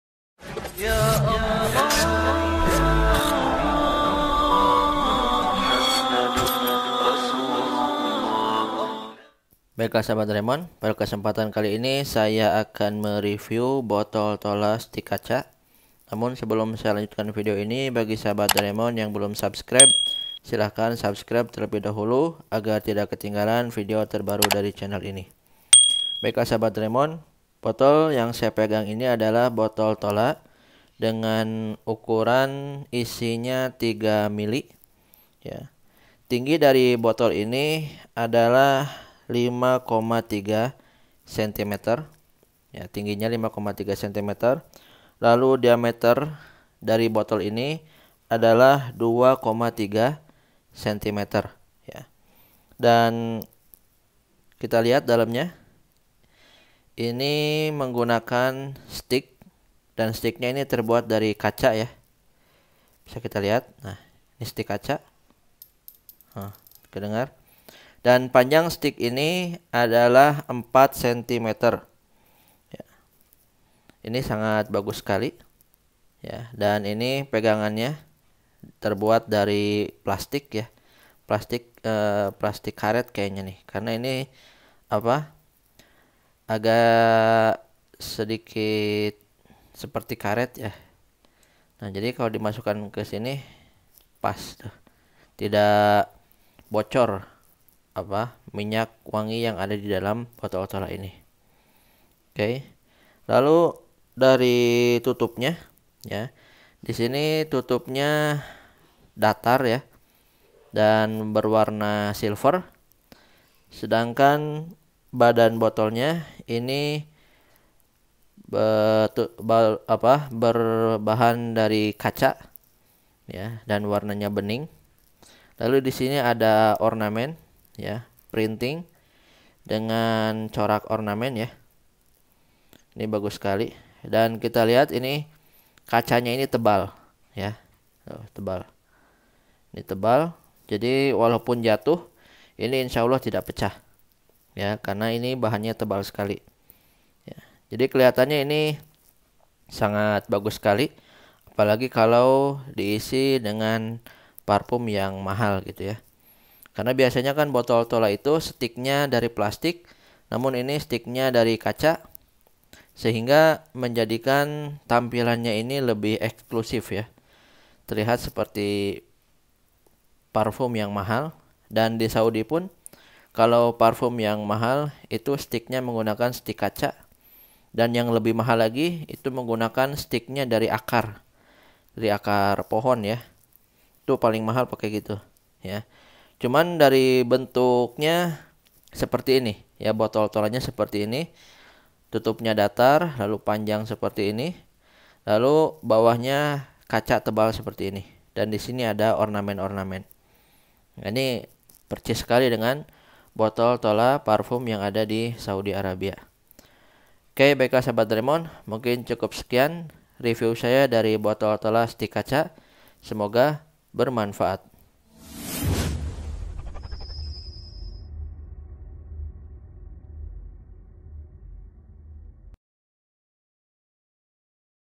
BOTOL TOLA STIK KACA Bikin Parfum Terlihat Mahal
Botol tola ukuran 3ml dengan tampilan eksklusif ditambah menggunakan stik kaca sehingga menjadikan parfum terlihat mahal.